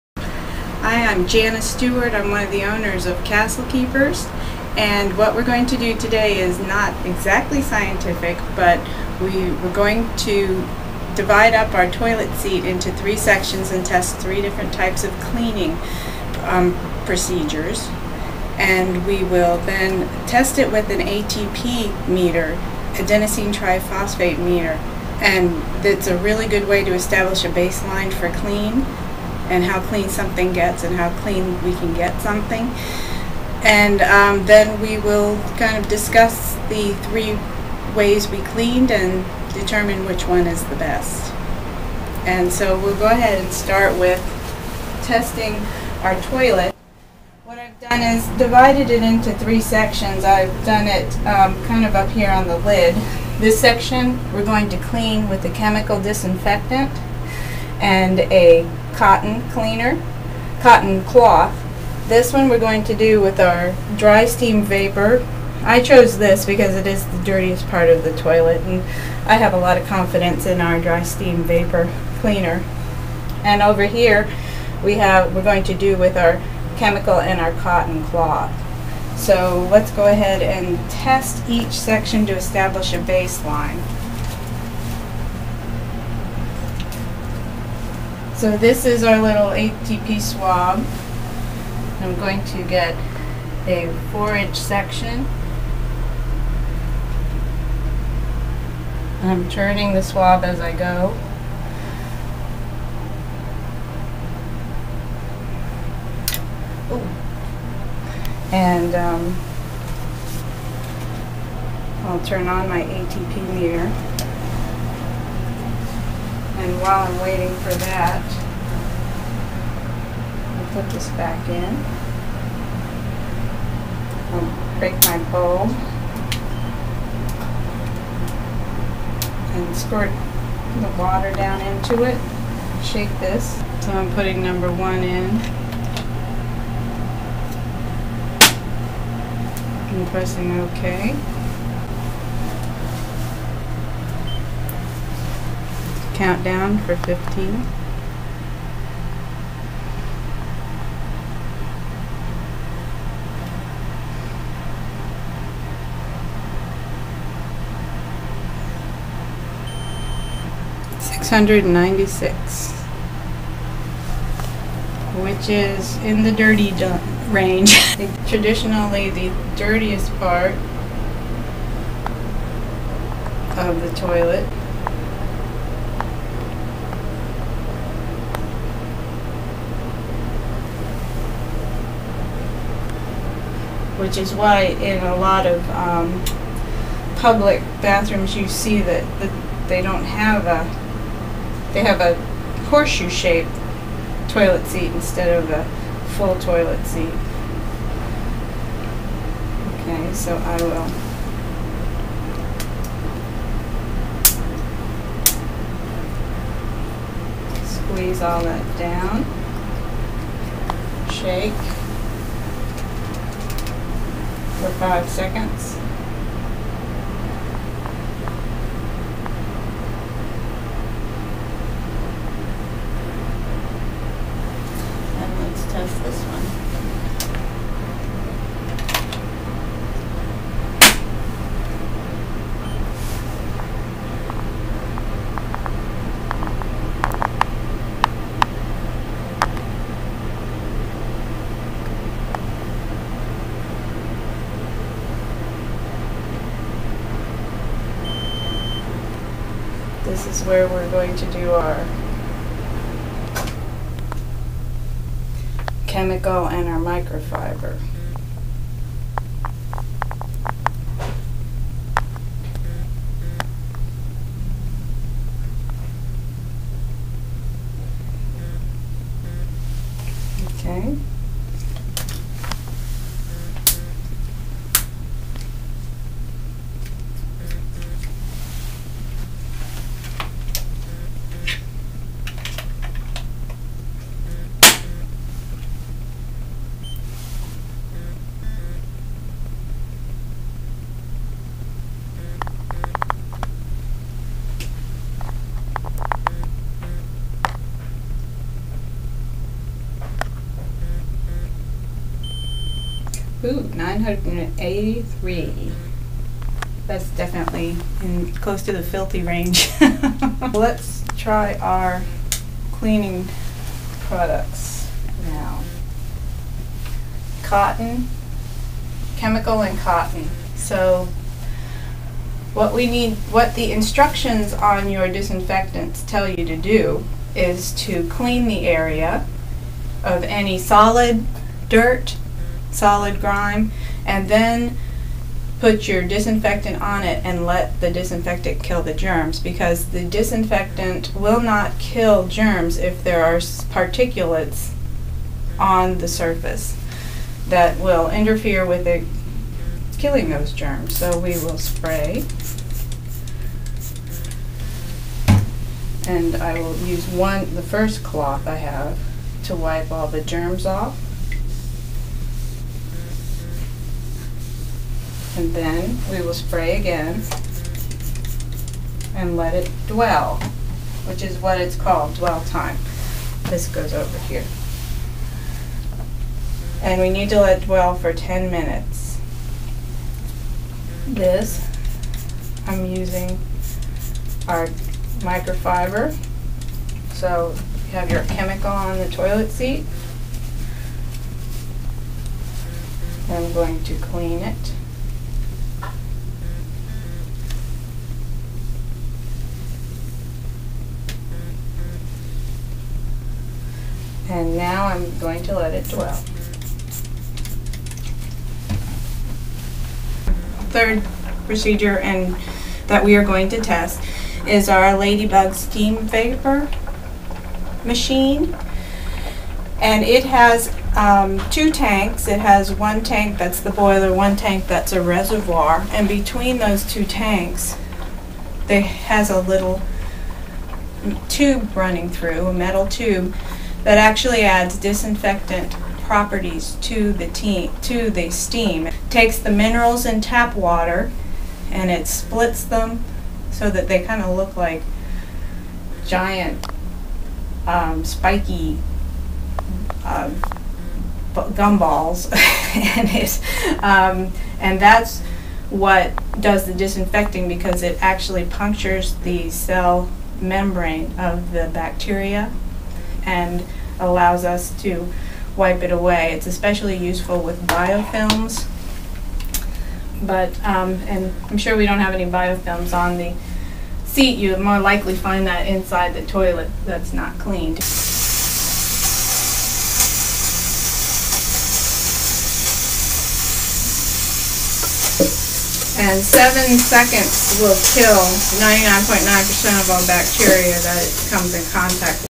How clean is your toilet? Part 1
Watch Janice Stewart, owner of Castle Keepers, test the parts of a toilet seat using an ATP meter, which measures adenosine triphosphate, the universal energy molecule found in all animal, plant, bacterial, yeast and mold cells. It's an objective way to test the cleanliness of a surface.